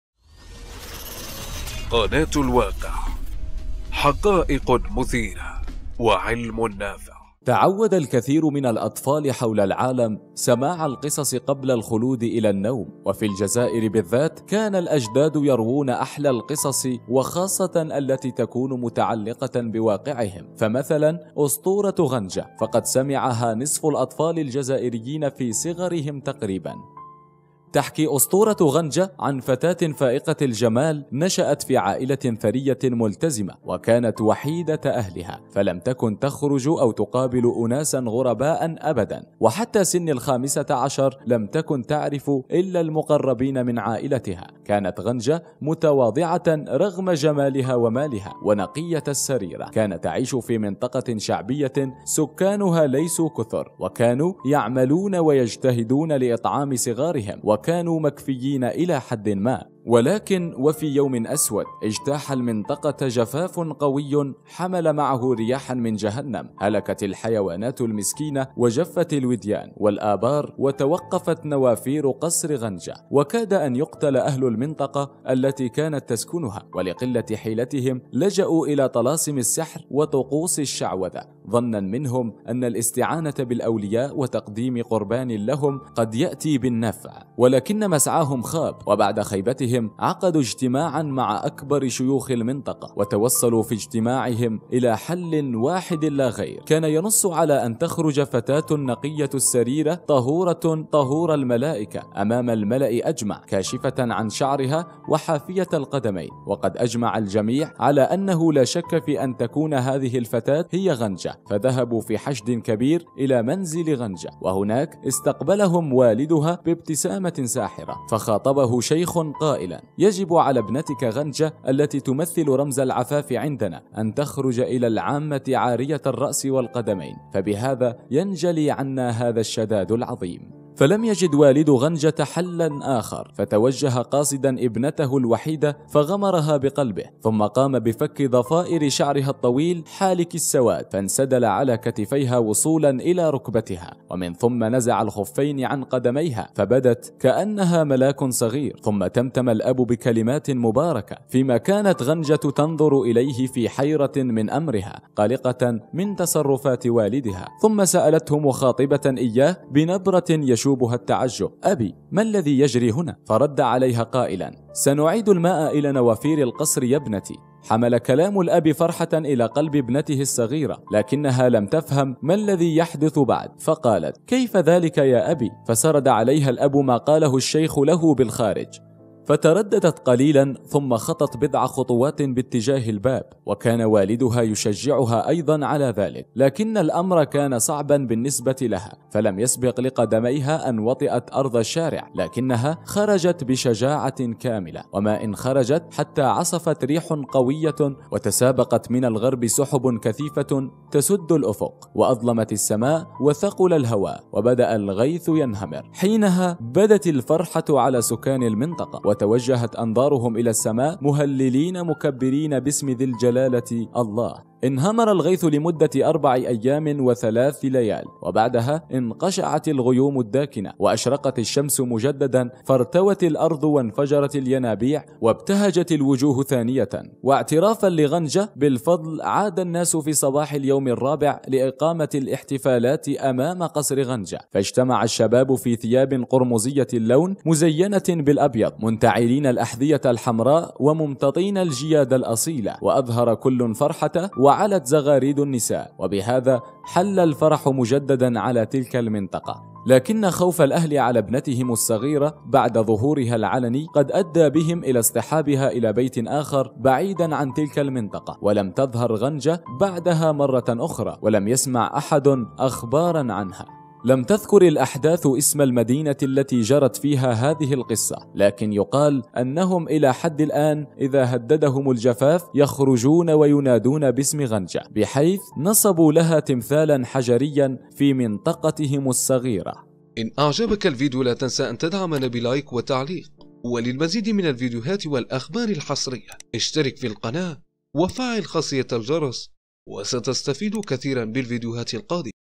غنجة الفاتنة التي لايعرفها إلا الجزائريين أجمل إمرأة في العالم
أجمل جميلات العالم ملكه جمال الكون تحكي اسطورة غنجة عن فتاة فائقة الجمال نشأت في عائلة ثرية ملتزمة وكانت وحيدة أهلها فلم تكن تخرج او تقابل أناس غرباء ابدا وحتى سن 15 لم تكن تعرف الى المقربون من عائلتها...أجمل امرأة لعام